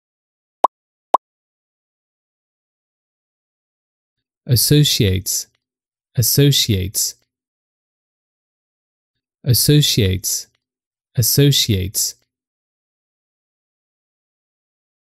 How to Pronounce Associates
In this video we'll show you how to correctly pronounce:  Associates.